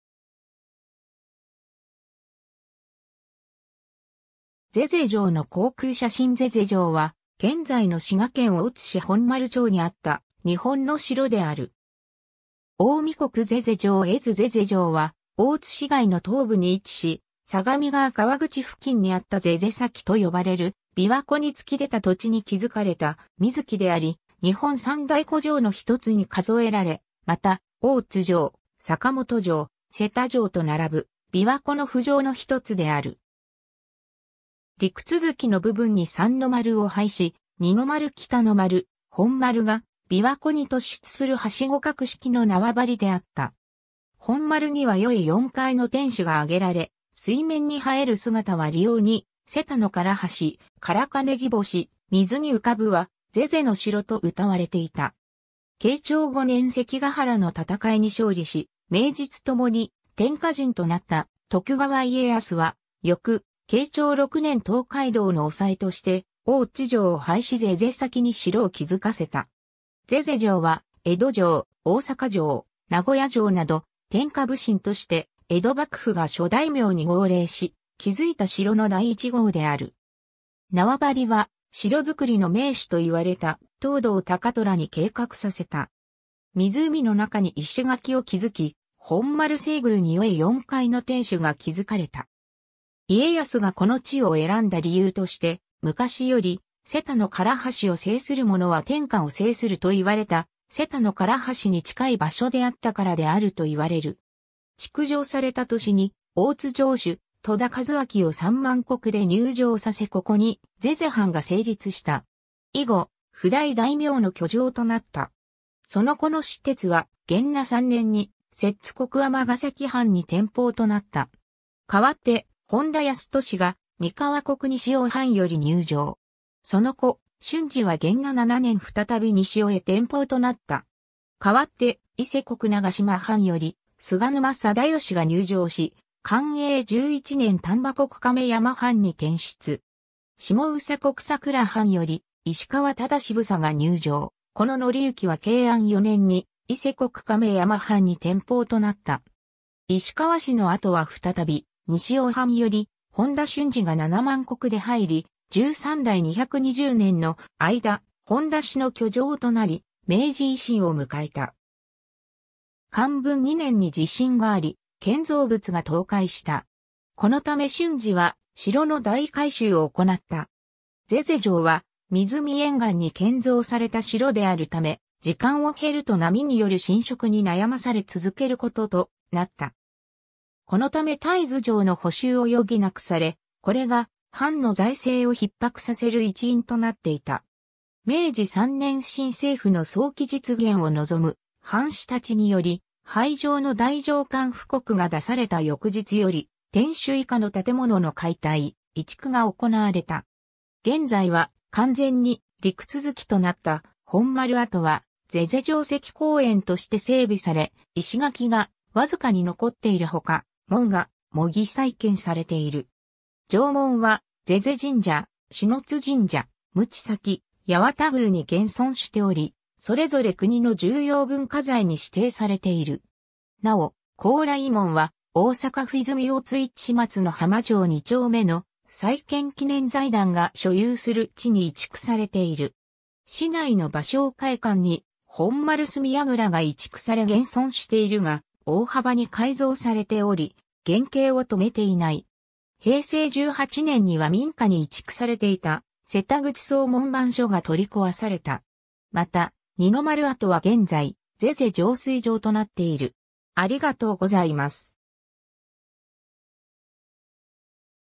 膳所城
膳所城の航空写真 （1982年撮影・国土航空写真） 膳所城（ぜぜじょう）は、現在の滋賀県大津市本丸町にあった日本の城である。
 近江国膳所城絵図 膳所城は大津市街の東部に位置し、相模川河口付近にあった膳所崎と呼ばれる琵琶湖に突き出た土地に築かれた水城であり、日本三大湖城の一つに数えられ、また大津城、坂本城、瀬田城と並ぶ「琵琶湖の浮城」の一つである。
陸続きの部分に三の丸を配し、二の丸・北の丸・本丸が琵琶湖に突出する梯郭式の縄張りであった。
本丸には4重4階の天守が上げられ、水面に映える姿は里謡に「瀬田の唐橋、唐金擬宝珠（からかねぎぼし）、水に浮かぶは膳所の城」と謡われていた。
 慶長5年（1600年）関ヶ原の戦いに勝利し名実共に天下人となった徳川家康は、翌、慶長6年（1601年）東海道の押さえとして、大津城を廃し膳所崎に城を築かせた。
膳所城は江戸城、大坂城、名古屋城など天下普請として江戸幕府が諸大名に号令し築いた城の第一号である。
縄張りは城造りの名手と言われた藤堂高虎に計画させた。
湖の中に石垣を築き、本丸西隅に4重4階の天守が築かれた。
家康がこの地を選んだ理由として、昔より「瀬田の唐橋を征するものは天下を征する」と言われた瀬田の唐橋に近い場所であったからであると言われる。
 築城された年に大津城主戸田一西を3万石で入城させここに膳所藩が成立した。
以後、譜代大名の居城となった。
その子の氏鉄は元和3年（1617年）に摂津国尼崎藩に転封となった。
 代わって本多康俊が三河国西尾藩より入城。
その子、俊次は元和7年（1621年）再び西尾へ転封となった。
代わって、伊勢国長島藩より菅沼定芳が入城し寛永11年（1634年）丹波国亀山藩に転出。
下総国佐倉藩より石川忠総が入城、子の憲之は慶安4年（1651年）に伊勢国亀山藩に転封となった。
 石川氏の後は再び西尾藩より本多俊次が7万石で入り、13代220年の間、本多氏の居城となり明治維新を迎えた。
 寛文2年（1662年）に地震があり、建造物が倒壊した。
このため俊次は城の大改修を行った。
 膳所城は湖沿岸に建造された城であるため、時間を経ると波による浸食に悩まされ続けることとなった。
このため絶えず城の補修を余儀なくされ、これが藩の財政を逼迫させる一因となっていた。
 明治3年（1870年）新政府の早期実現を望む藩士達により、廃城の太政官布告が出された翌日より天守以下の建物の解体・移築が行われた。
 現在は完全に陸続きとなった本丸跡は「膳所城跡公園」として整備され、石垣がわずかに残っているほか、門が模擬再建されている。
城門は膳所神社（本丸大手門）、篠津神社（北大手門）、鞭崎八幡宮（南大手門）に現存しており、それぞれ国の重要文化財に指定されている。
なお、高麗門は大阪府泉大津市松之浜町2丁目の「細見記念財団」が所有する地に移築されている。
市内の芭蕉会館に本丸隅櫓が移築され現存しているが、大幅に改造されており原形を留めていない。
 平成18年（2006年）には民家に移築されていた瀬田口総門番所が取り壊された。
また、二の丸跡は現在、膳所浄水場となっている。